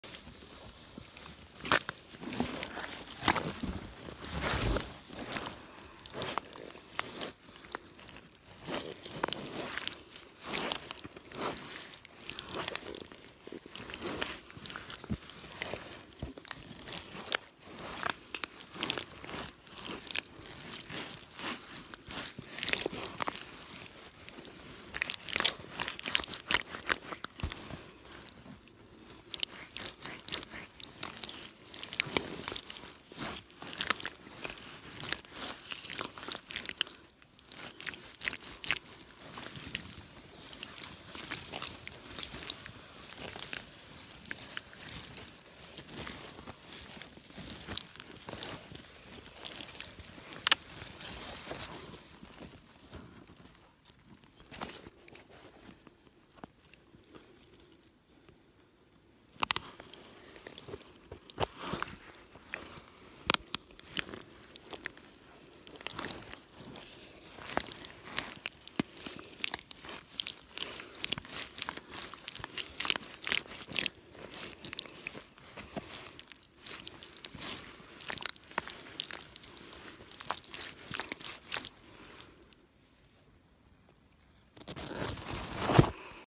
Muddy rubber boots.（Mitsuuma）
森林を散歩していたら深いぬかるみに遭遇！